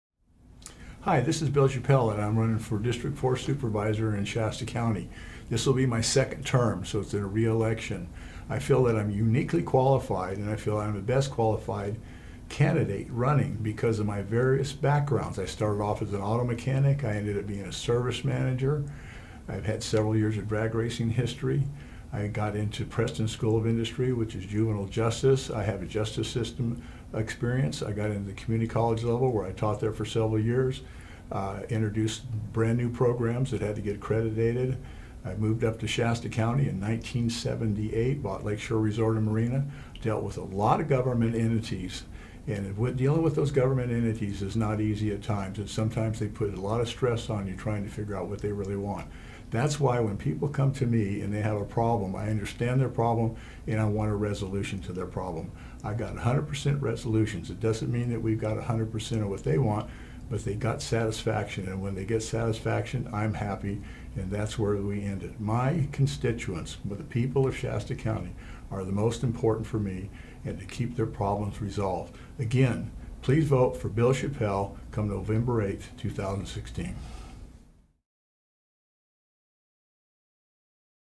Bill Schappell Statement
Bill Schappell is running for District 4 Shasta County Supervisor.